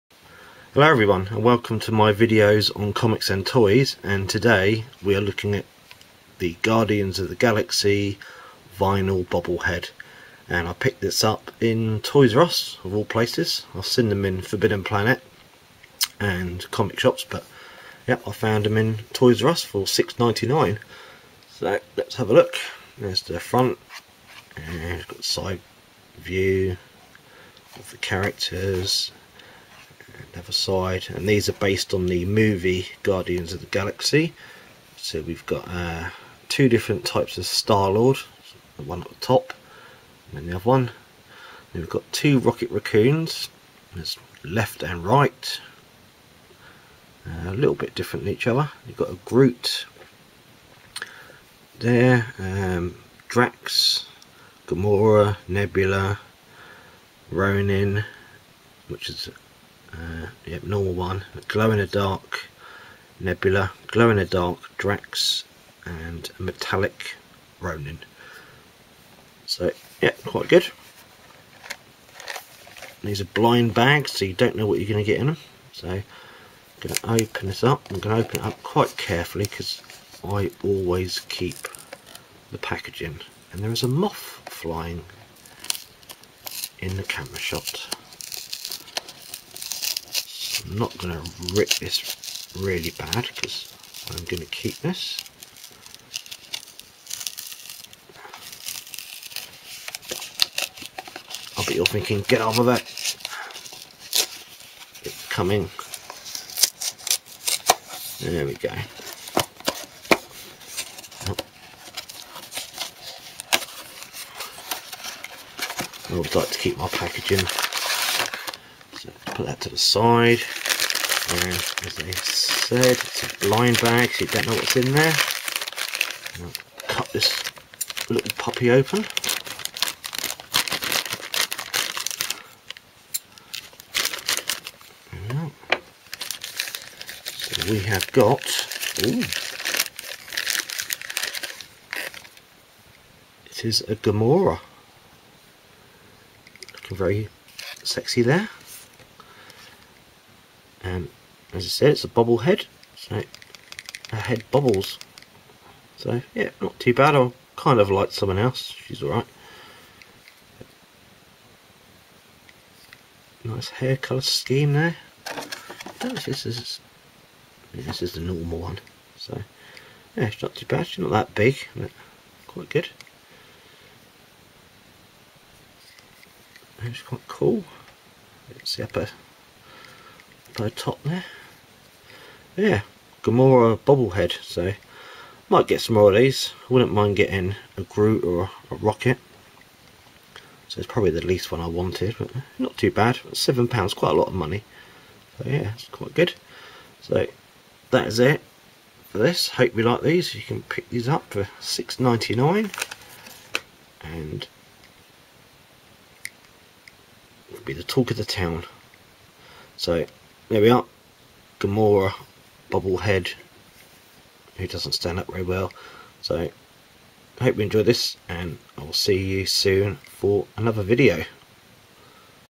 Guardians of the Galaxy Bobble Head.
A look at the new Guardians of the Galaxy blind bag bobble head.